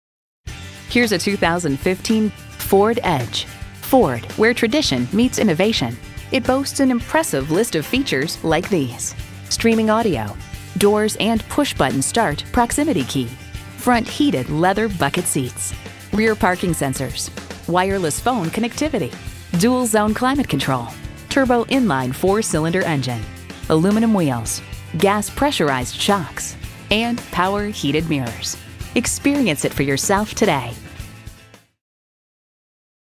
Used 2015 Ford Edge Wausau, WI
Year: 2015
Make: Ford
Model: Edge
Trim: SEL
Engine: EcoBoost 2.0L I4 GTDi DOHC Turbocharged VCT
Transmission: 6-Speed Automatic
Color: Gray
Interior: Ebony
Mileage: 49248
Stock #: P6843
VIN: 2FMTK3J99FBB89868
Gray 2015 Ford Edge SEL 4D Sport Utility LEATHER SEATS, Electronic Stability Control, Equipment Group 201A, Exterior Parking Camera Rear, Leather-Trimmed Heated Bucket Seats, Low tire pressure warning, Premium 9 Speaker Audio, Speed-Sensitive Wipers, SYNC w/MyFord Touch, Wheels: 18" Aluminum Painted Sparkle Silver. EcoBoost 2.0L I4 GTDi DOHC Turbocharged VCT FWD 30/20 Highway/City MPGbrbrAwards:br * 2015 IIHS Top Safety Pick * 2015 KBB.com Brand Image Awards

Address:
2727 North 20th Ave